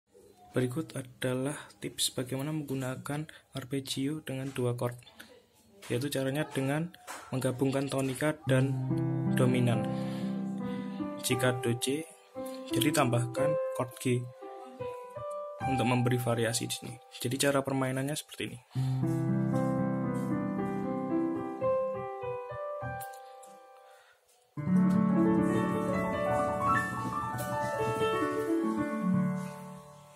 #1 Arppegiato
Bagaimana cara mengembangkan permainan arppegiato? Salah satunya dengan menggabungkan Tonic & Dominant. Simak video berikut ya🎹